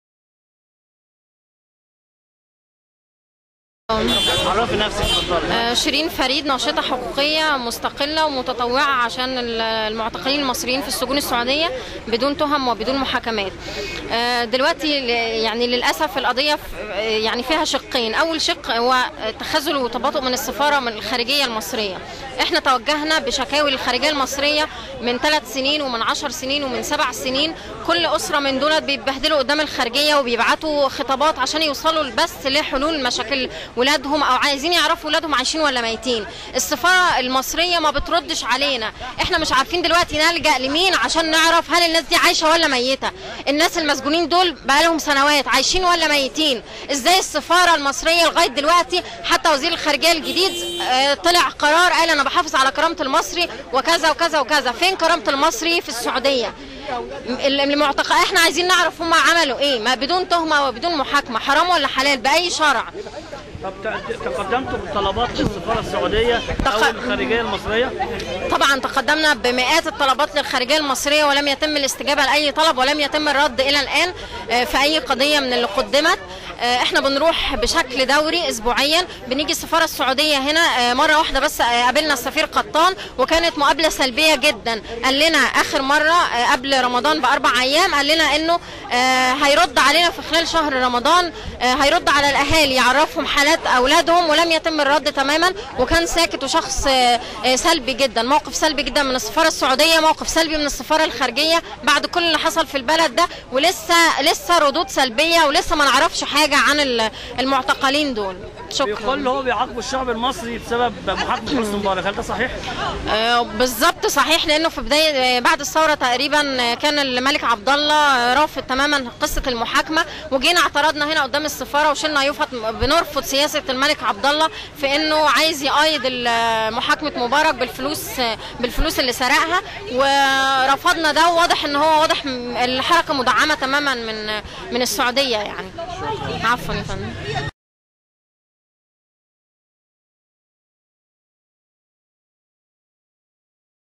ناشطة حقوقية متطوعة مع أهالى المعتقلين أمام السفارة
ناشطة حقوقية مستقلة متطوعة بجانب أهالى المعتقلين بالسعودية أمام السفارة السعودية